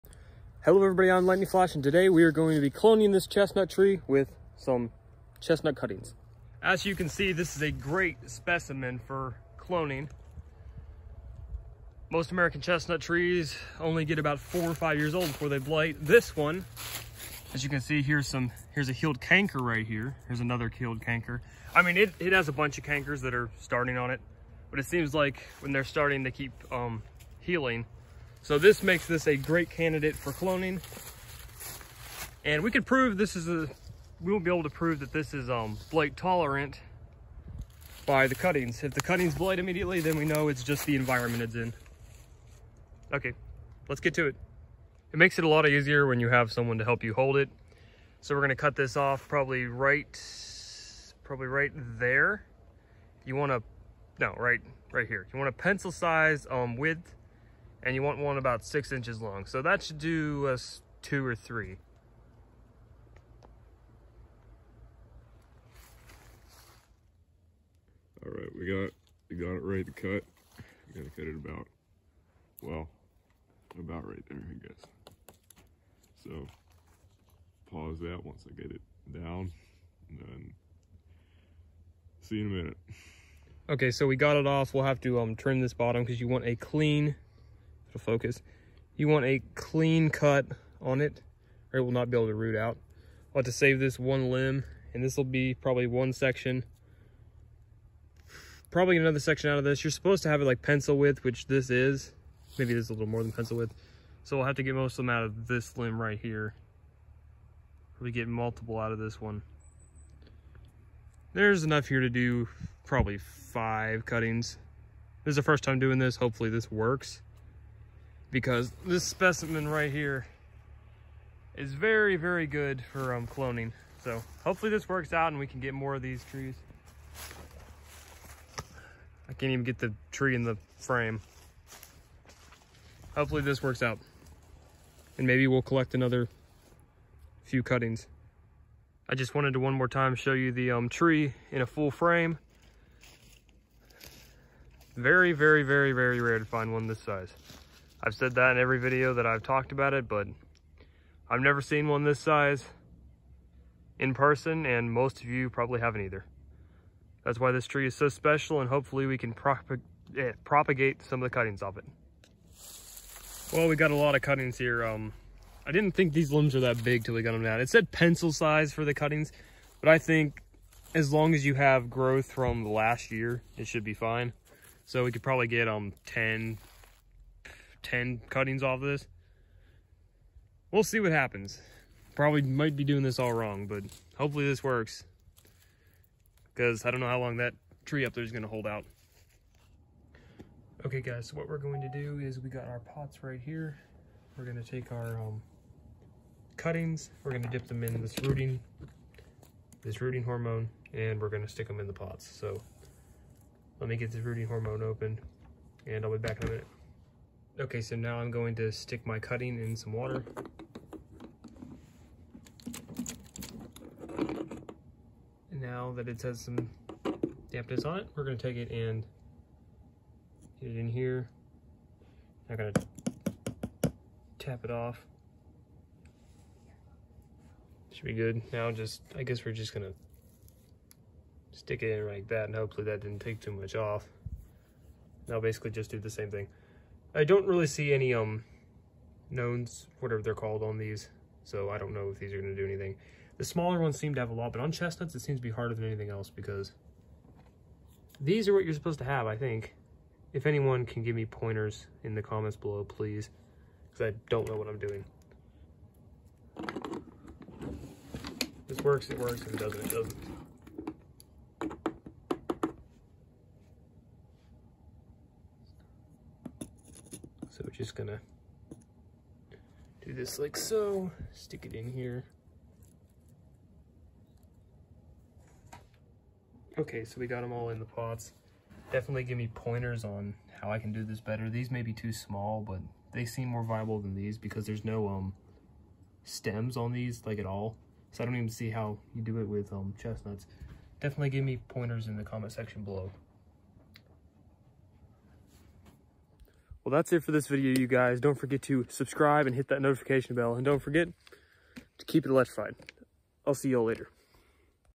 Cloning a Surviving American Chestnut Tree (From Cuttings)
Today we try our luck at cloning a surviving American chestnut from cuttings.